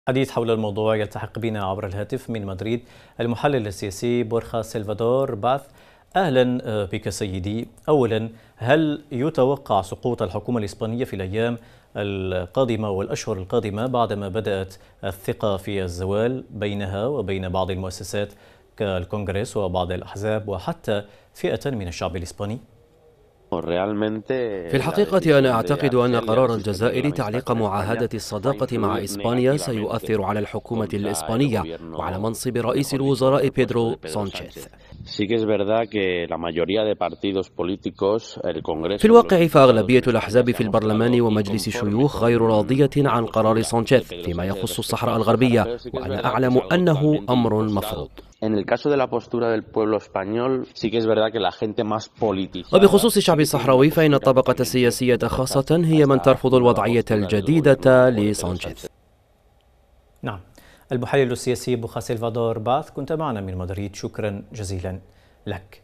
محلل اسباني: قرار الجزائر تعليق اتفاقية التعاون سيؤثر على اسبانيا
معاهدة الصداقة وحسن جوار والتعاون بين الجزائر واسبانيا
الجزائر ترد على الاتحاد الأوربي
رد الخارجية الجزائرية على مفوضية الاتحاد الأوربي